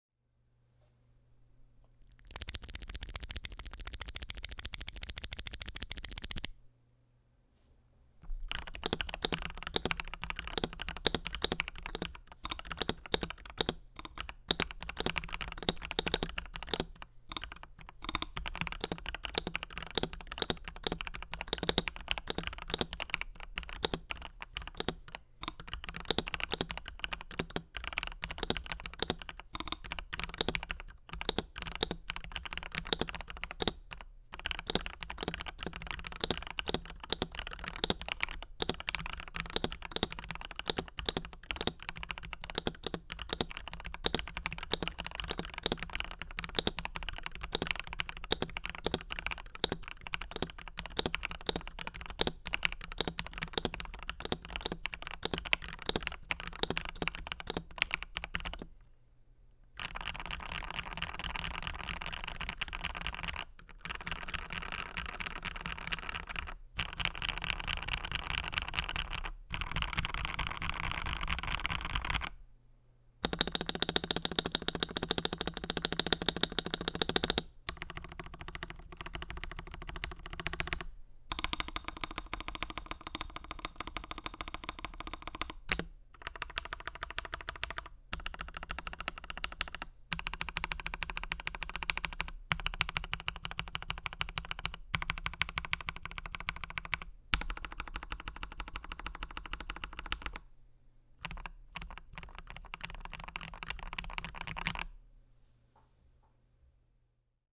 ⌨️ FL.eSports FL750 - Kailh ICE Mint typing sounds
⌨️ Specs
• Keyboard : FL.eSports FL750
• Plate : stainless steel
• Keycaps : FSA PBT doubleshot
• ‎Switches : Kailh POM ICE Mint
• Stabilizers : Plate mounted (factory pre-lubed)
• Keyboard mount type : isolation mount

Mic : ISK BM-800
Audio interface : Roland Rubix24
Mic stand : Thronmax Zoom S3